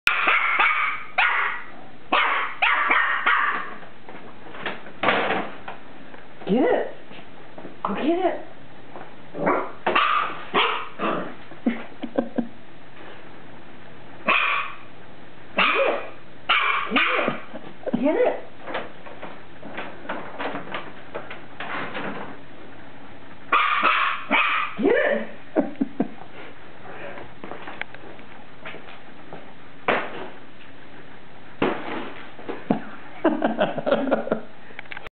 Puppy Emma vs paper bag.MOV
Norfolk Terrier Emma does not like paper bags